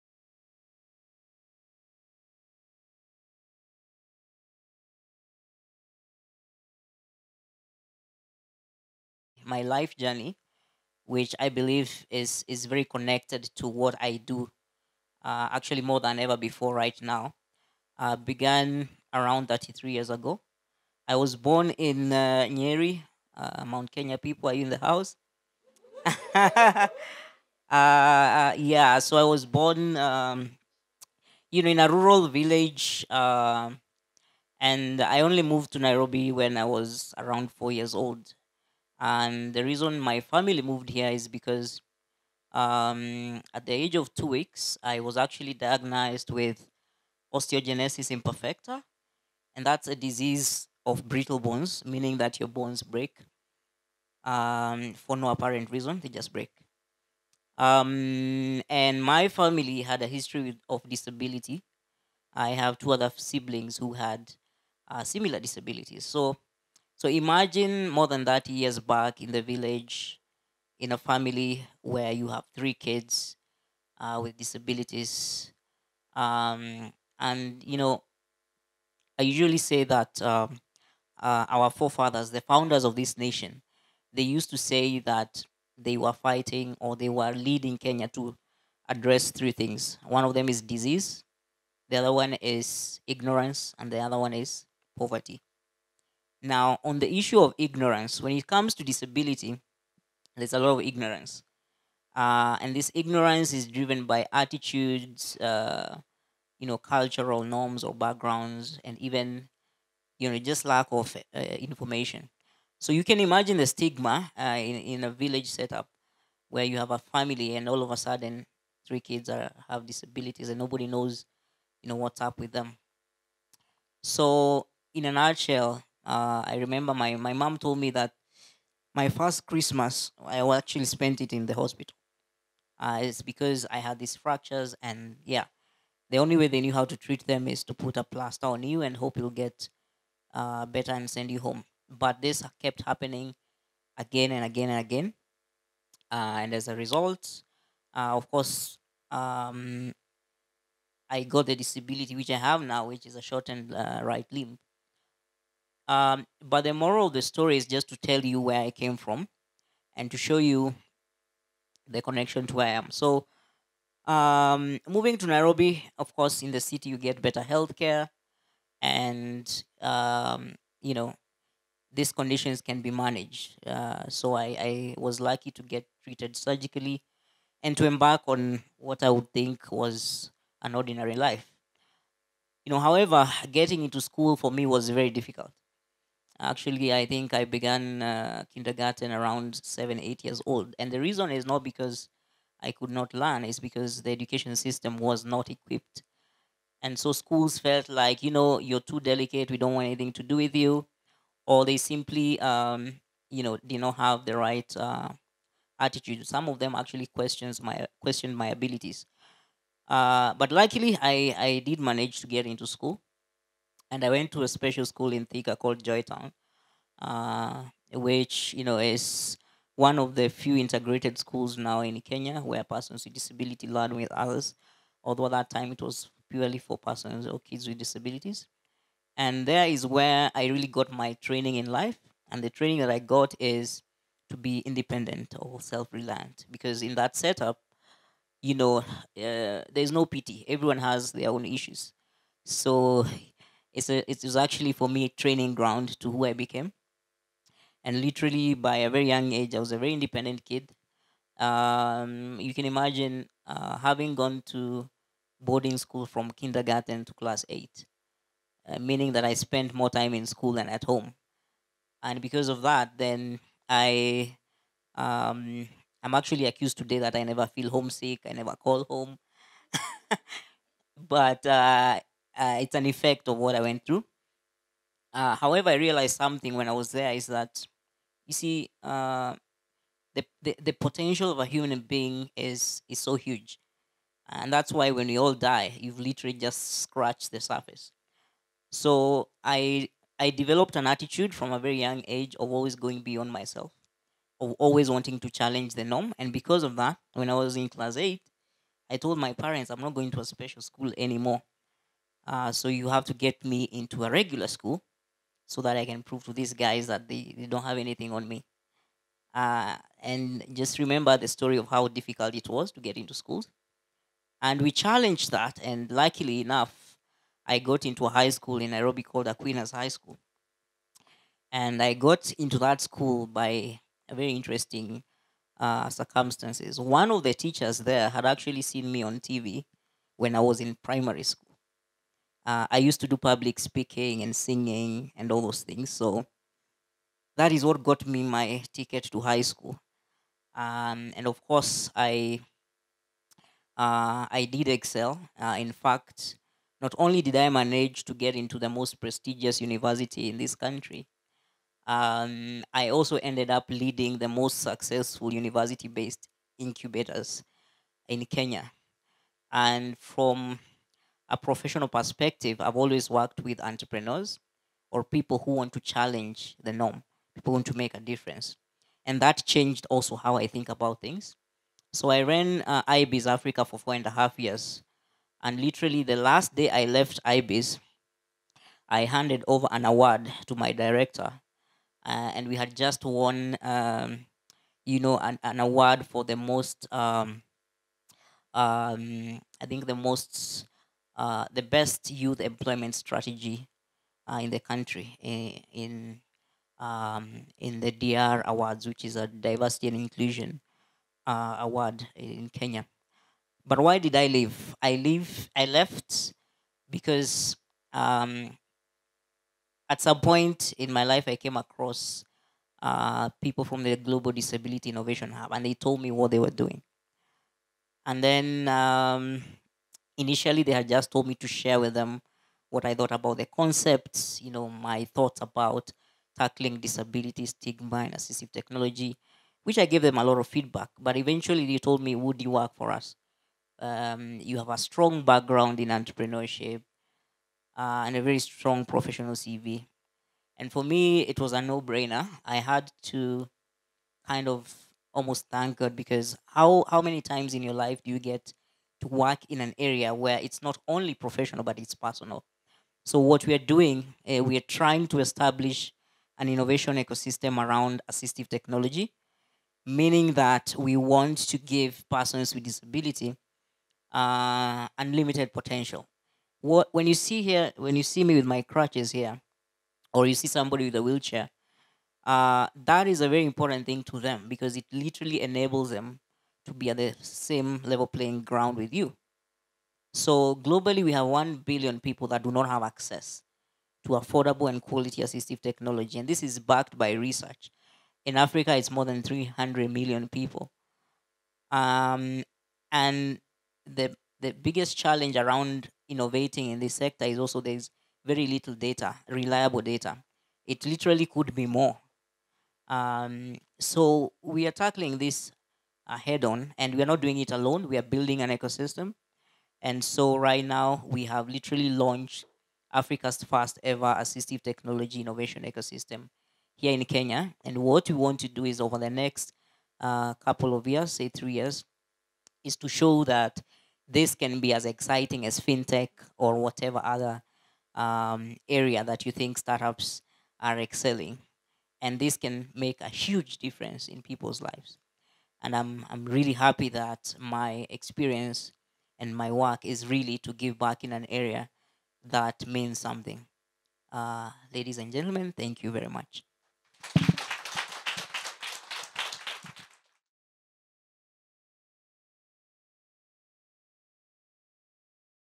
Walk Your Talk - Bernard Chiira
Bernard Chiira shares his own inspirational journey of making an impact whilst living with disability and his campaign to ensure the needs and concerns of  the disabled are catered to in Africa.